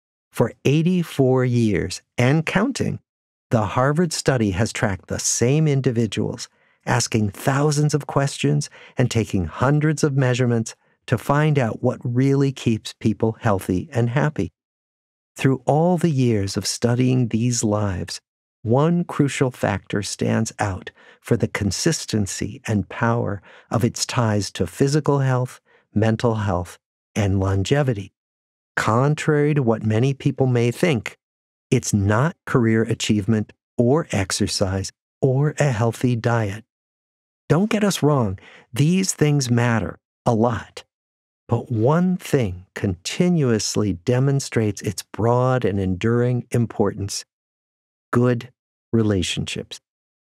Listen to THE GOOD LIFE
Sound on! Listen to an excerpt from the audiobook edition of THE GOOD LIFE, written and read by Robert Waldinger, M.D. and Marc Schulz, Ph.D.

What makes for a happy life, a fulfilling life? A good life? According to the directors of the Harvard Study of Adult Development, the longest scientific study of happiness ever conducted, the answer to these questions may be closer than you realize.